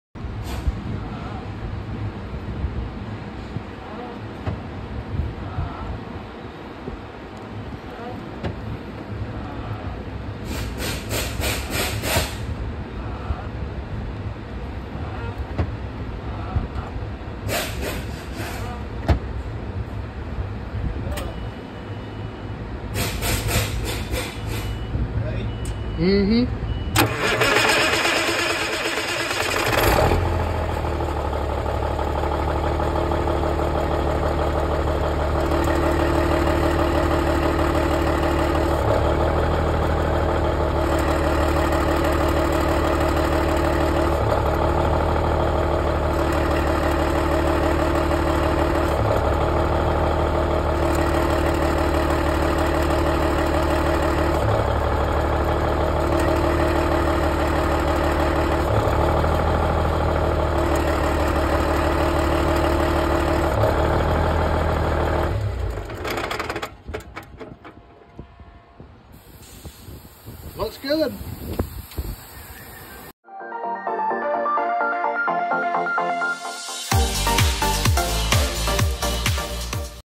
Eaton Heavy duty manual transmission clutch engagement
Just a quick video for visual reference on what a properly adjusted Eaton clutch looks like. You can see how the throw out bearing makes contact with the clutch brake and stops the input shaft of the transmission. When this happens it allows you to put your truck in gear.